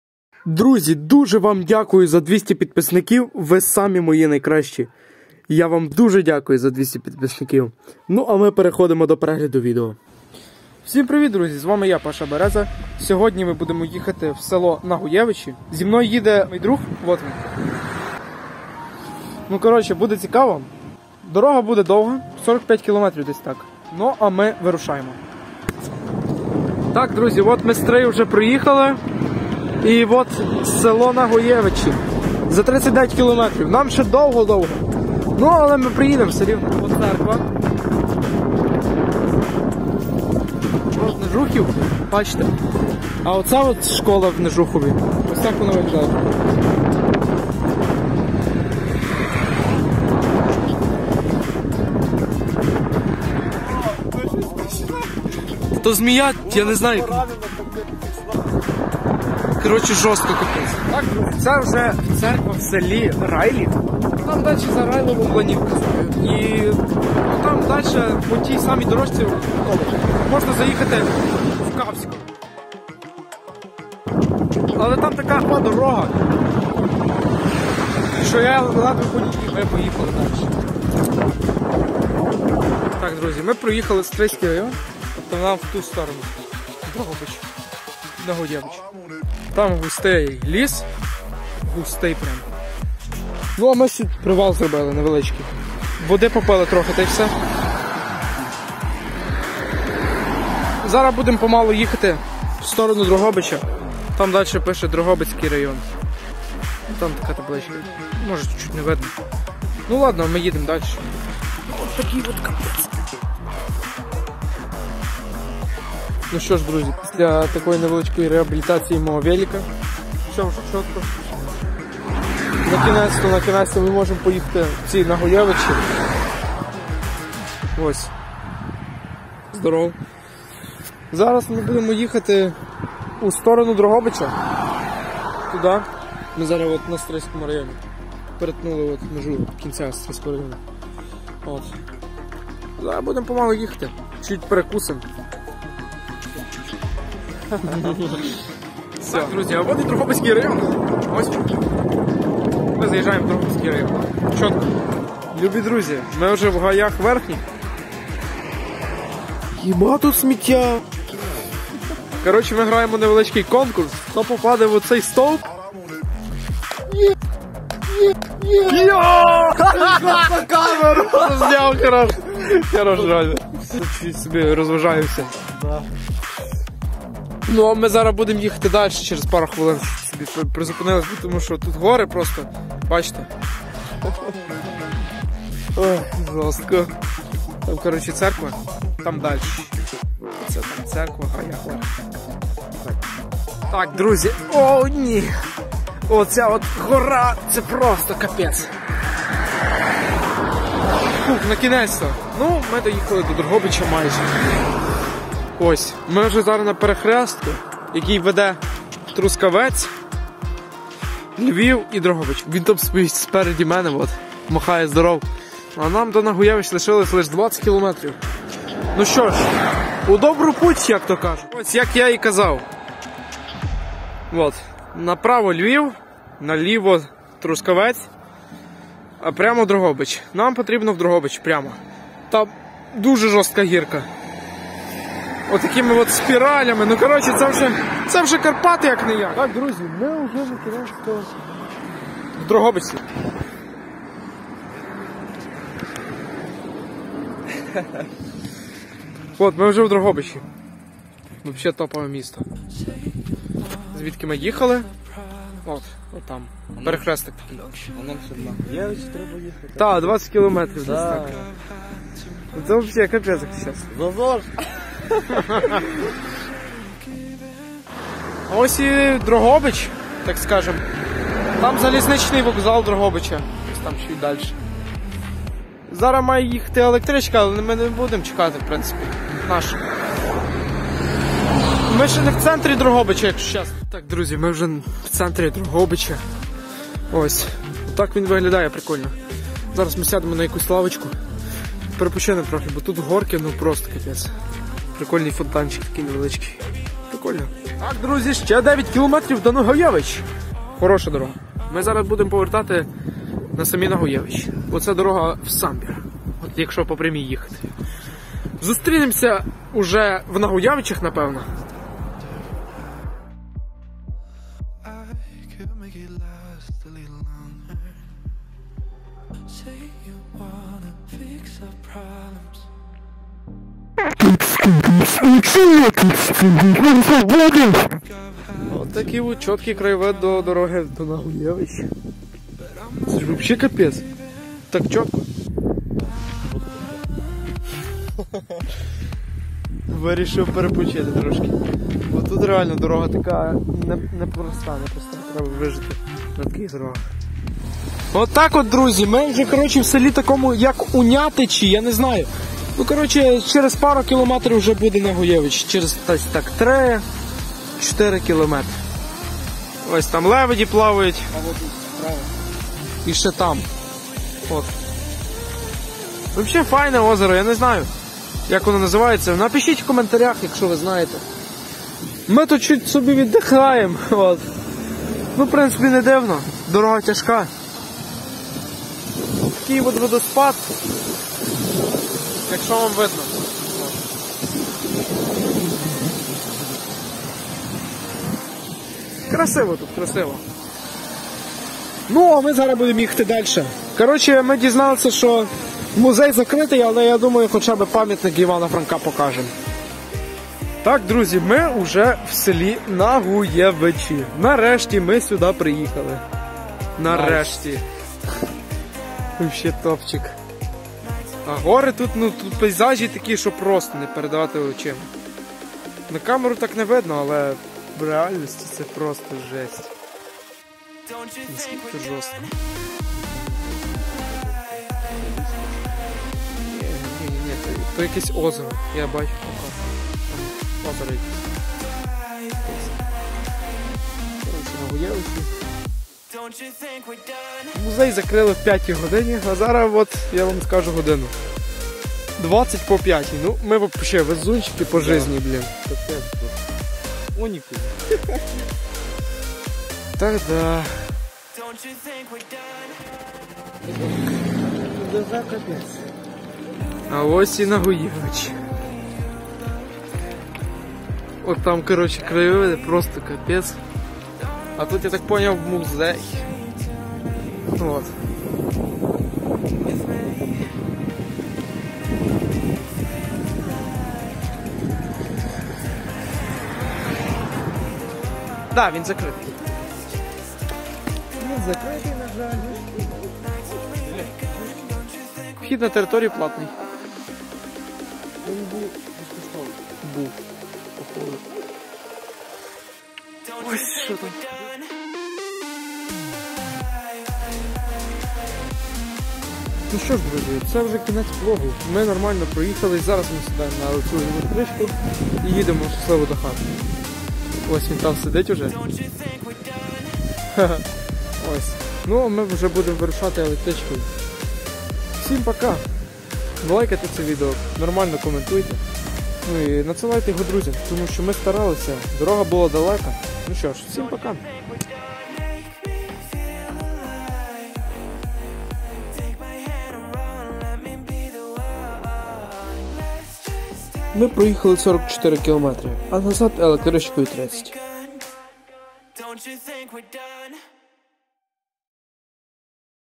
З'їздив з другом до м. Дрогобич та с. Нагуєвичі! PASHA VLOGS №4 (2 season)
Нарешті!!! Напевно ви дочекались випуску нового відео! Дуже дякую любі друзі! Нас все більше і більше) Я думаю ми зможемо дойти до позначки у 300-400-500!
А тепер про влог:
Цього разу я вирішив поїхати не сам, а з другом! І ми вирішили поїхати до м. Дрогобич, а заодно до с. Нагуєвичі, саме там де народився відомий український письменник Іван Якович Франко. Детальніше у самому відео)